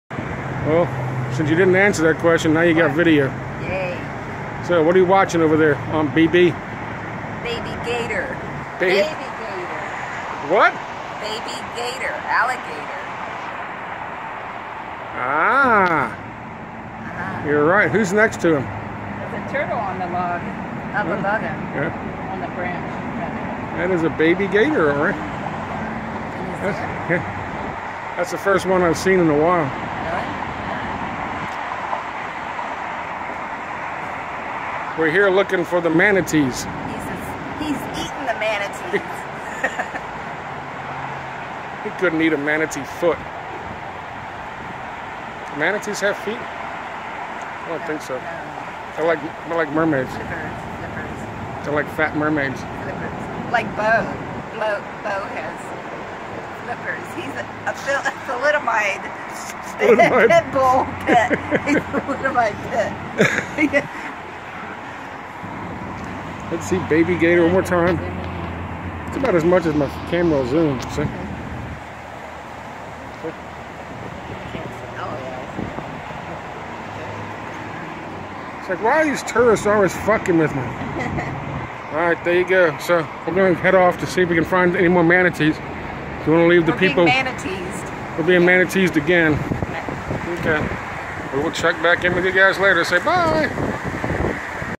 A Guy, a Gal & a Gator (Mirror from Scott Creighton's churchdog42 channel)
While retracing our steps in Sulphur Springs Park in the middle of Tampa, Florida, where we had found manatees last year, Scott Creighton (churchdog42 channel) & I found a baby alligator.